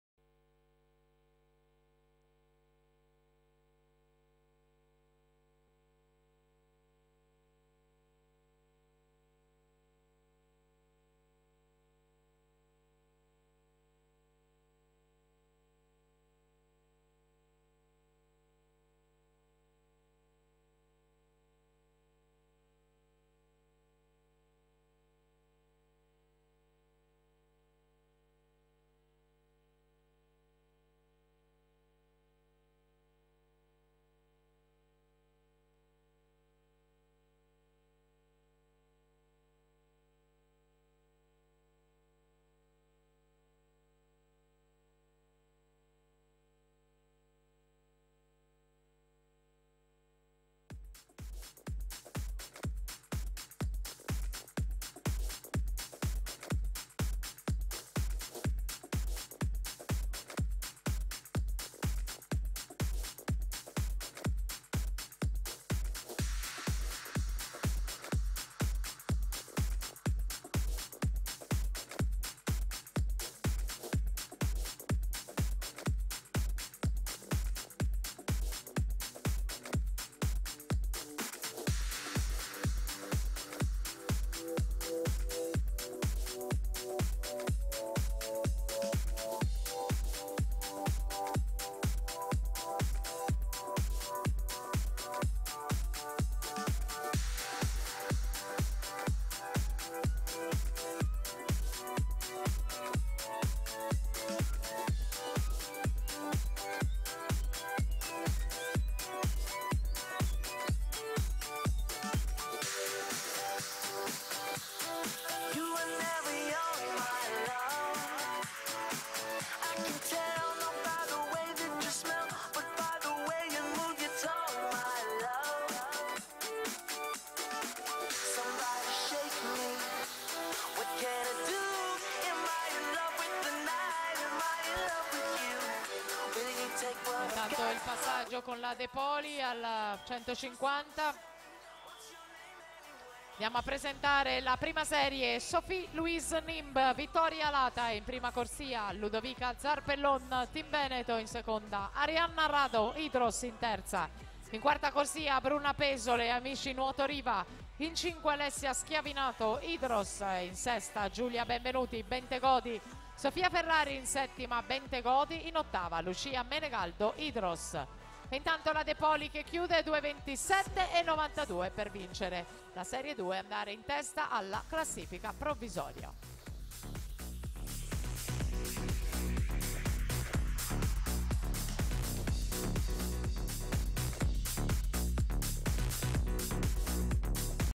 200 Misti Ass. Femminile Agoniste (Serie 2) - Meeting di Halloween 11Trofeo citta' di Rosa'
Gara di nuoto svolta a Rosa' alle 18:55 di Domenica 01/11/2015

Atleti partecipanti:
in corsia 1 GUARNATI VALENTINA (2000) della soc. TEAM LOMBARDIA NUOTO MGM iscritta con 02:34.00
in corsia 2 BELTRAME ALESSIA (2001) della soc. 3 PISCINE iscritta con 02:33.70
in corsia 3 ARTIOLI FIAMMETTA (2000) della soc. LEOSPORT  iscritta con 02:32.00
in corsia 4 NAPOLITAN CHIARA (2000) della soc. ACQUAMBIENTE iscritta con 02:31.50
in corsia 5 MAZZOTTA LAURA (2002) della soc. HYDROS iscritta con 02:32.00
in corsia 6 DE POLI ALESSANDRA (2002) della soc. TEAM VENETO iscritta con 02:32.60
in corsia 7 GIACON MARIA VITTORIA (2001) della soc. TEAM VENETO iscritta con 02:34.00
in corsia 8 TARANTELLO FRANCESCA (2002) della soc. PADOVANUOTO iscritta con 02:34.70